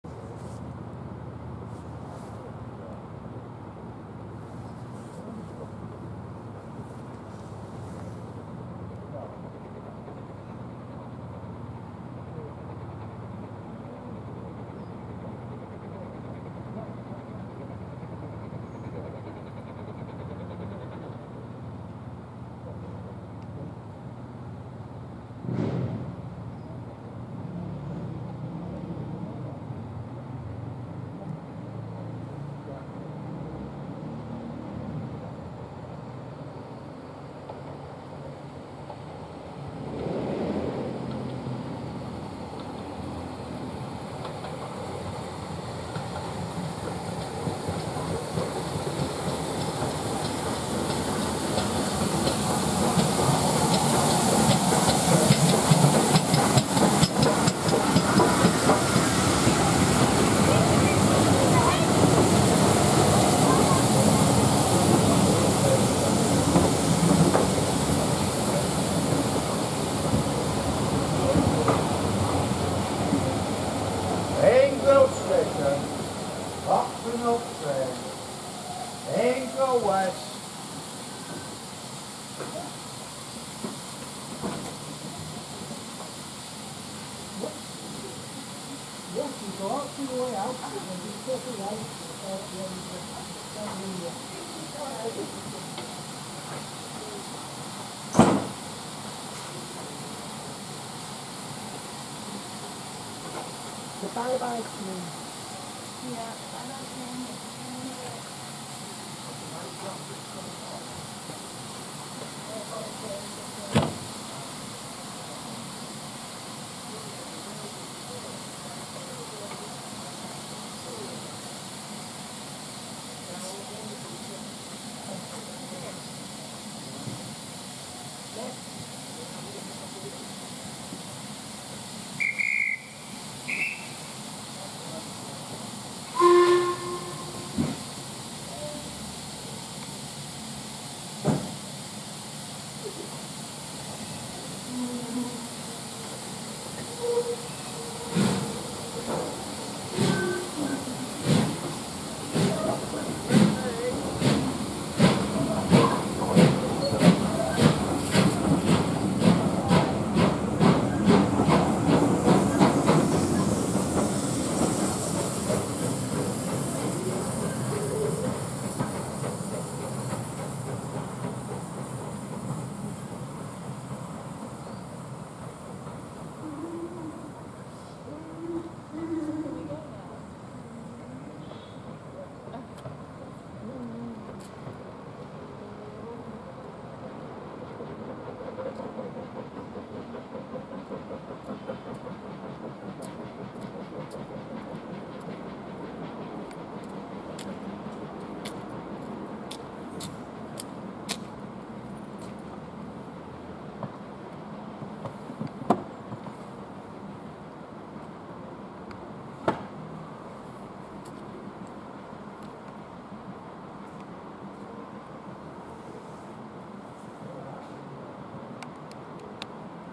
45212 at Ingrow West, at 2:22pm on Sunday 14th November 2021
London Midland and Scottish Railway Class 5MT ('Black 5') 4-6-0 No. 45212, at Ingrow West Railway Station, on the Keighley and Worth Valley Railway at 2:22pm on Sunday 14th November 2021.